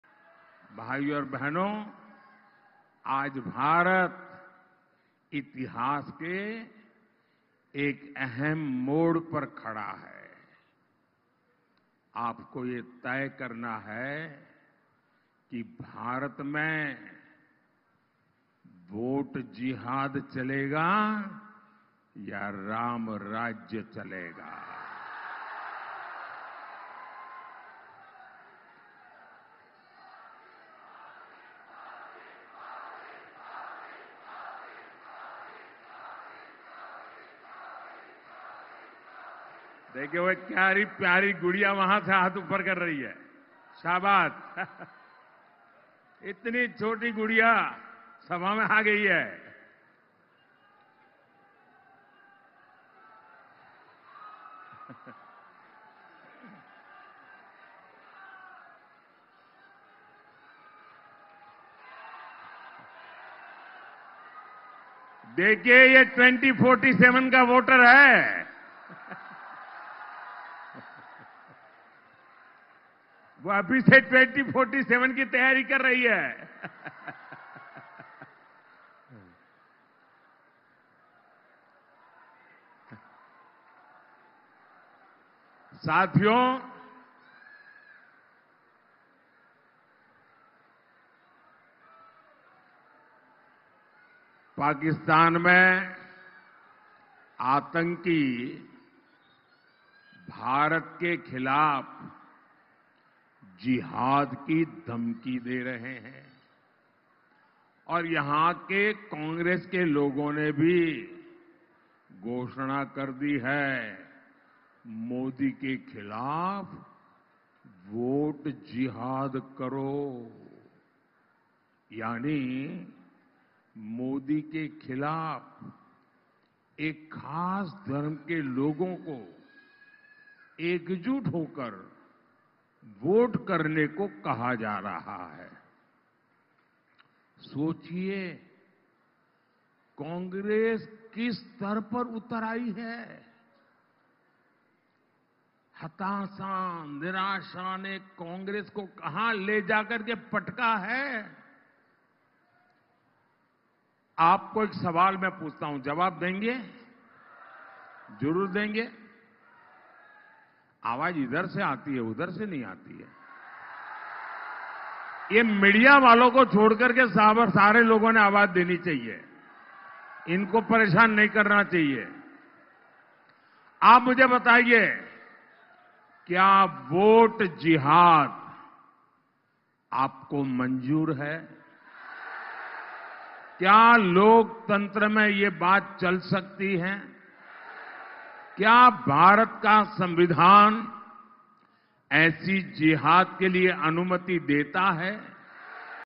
Today, India sees itself at a critical juncture of history: PM Modi
Addressing a rally in Khargone, Prime Minister Narendra Modi said that today, India sees itself at a critical juncture of history. He added that we all must collectively decide if we want a 'Ram Rajya' or a 'Vote Jihad'. He added that Pakistan has promised 'Jihad' against India and here Congress has promised 'Vote Jihad', so we all have to take a conscious decision on the same.